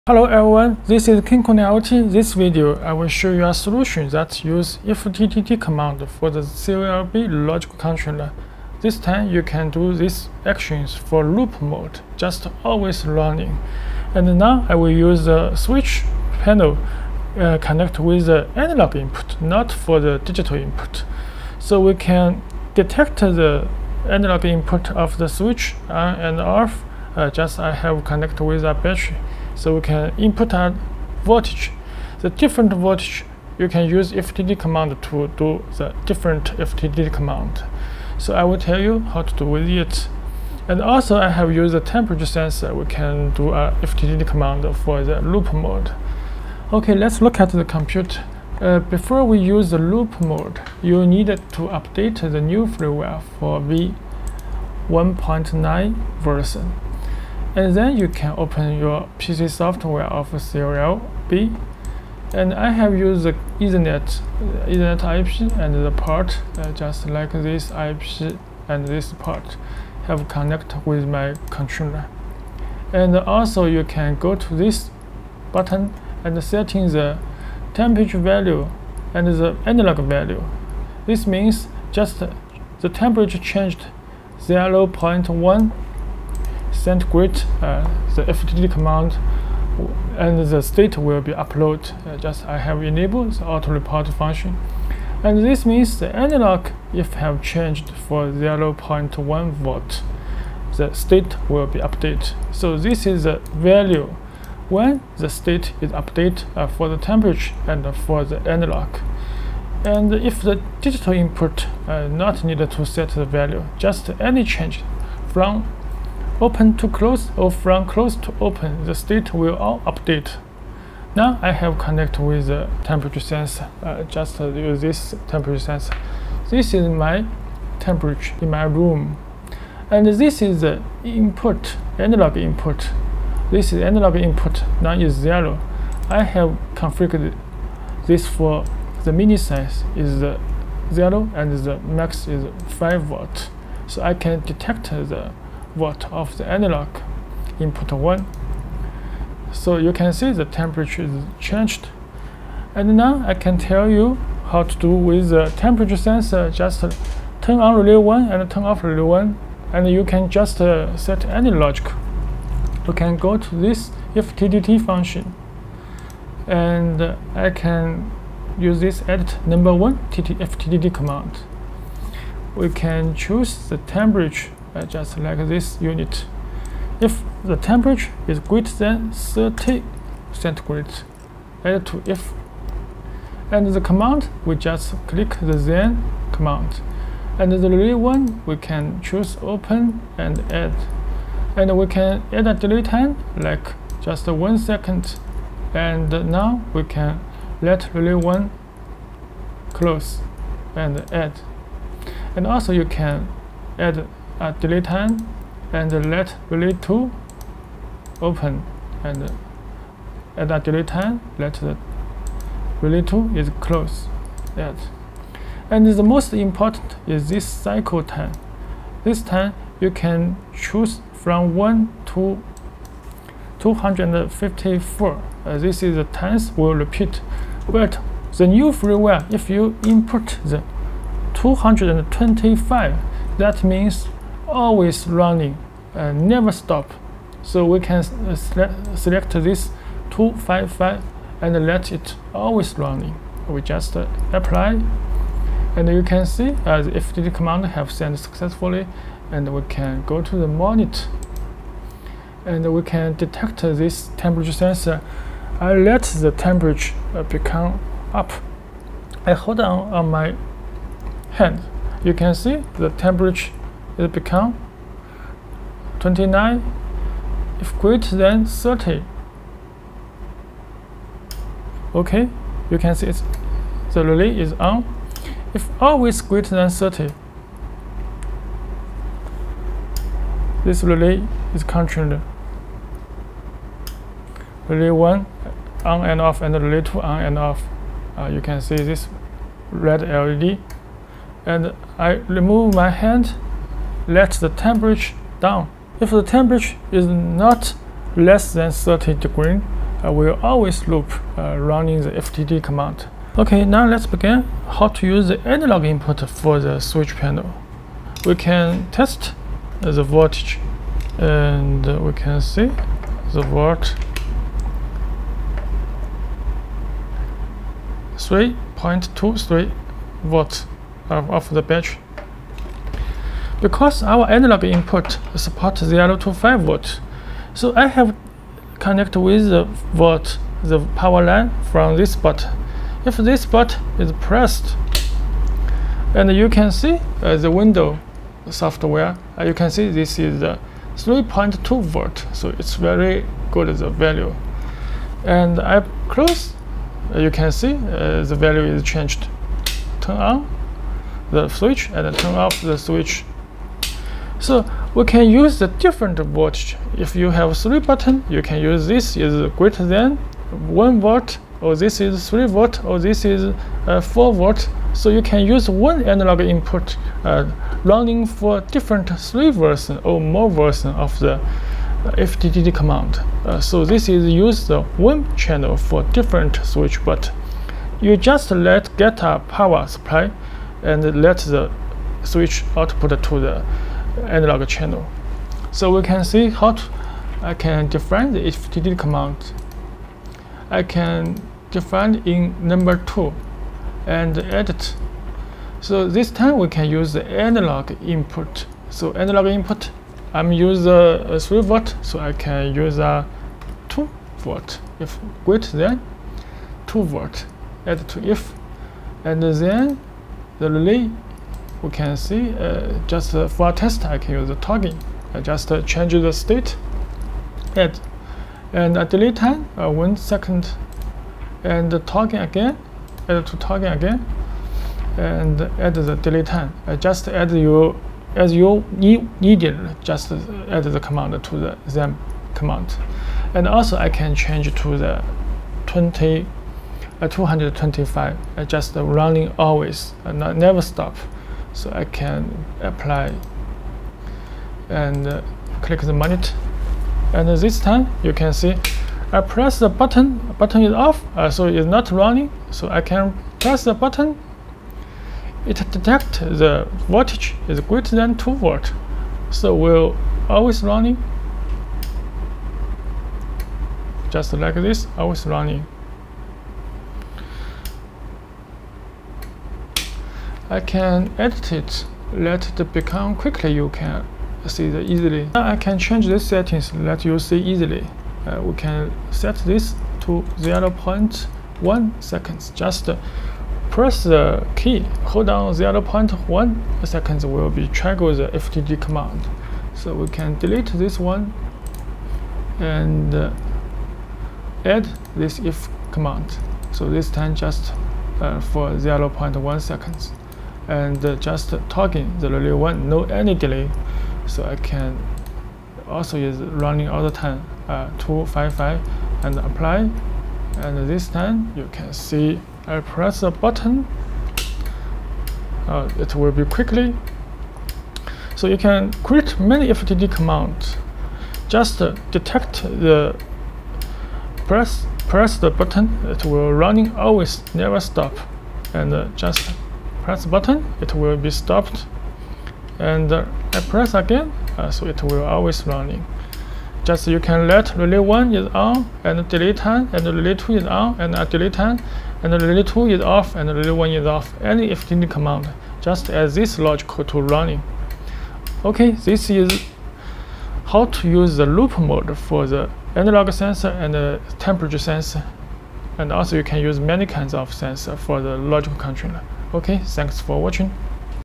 how to use temperature or analog input trigger relay in loop mode
new firmware update for KC868-COLB add the "loop" mode for automatic control relay.